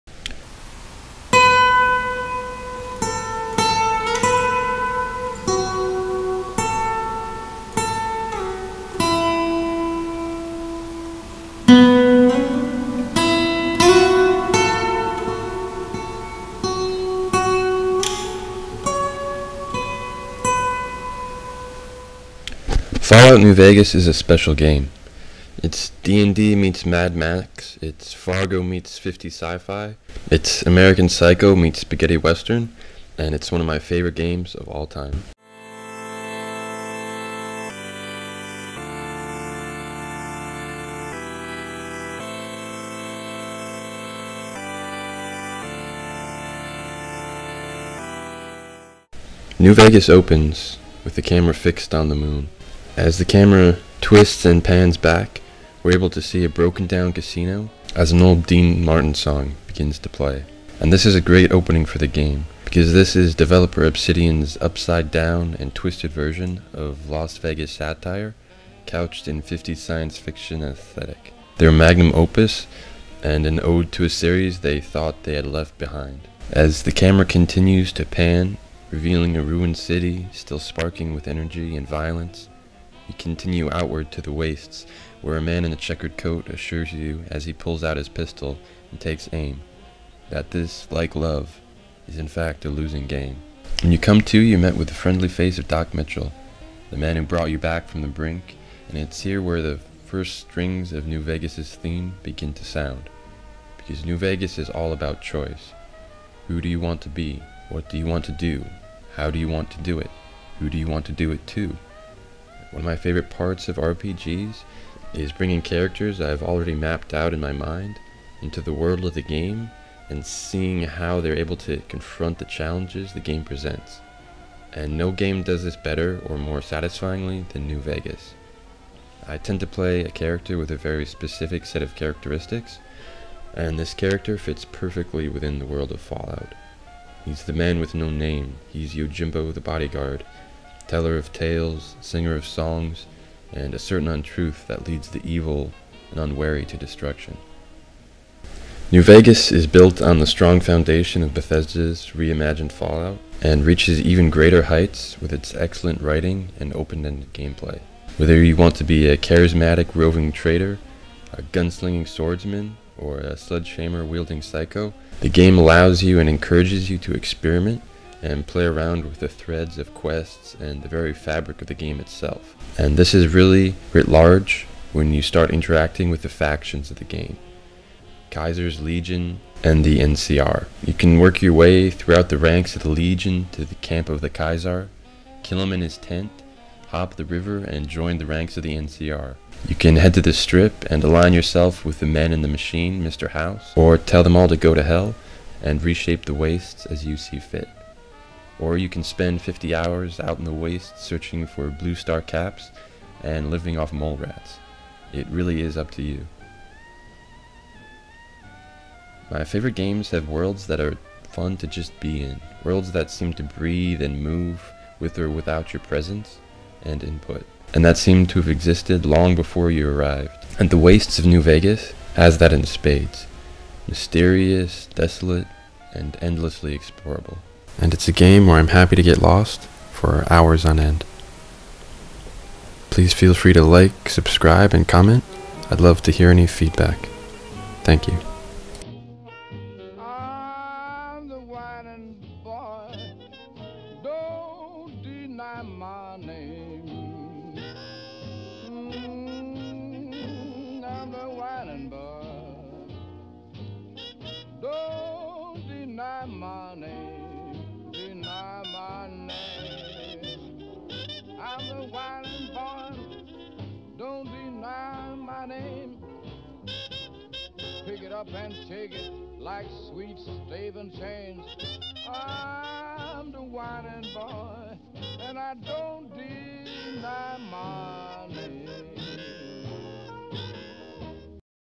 Why I love Fallout: New Vegas
The first in my new video series discussing games, writing and music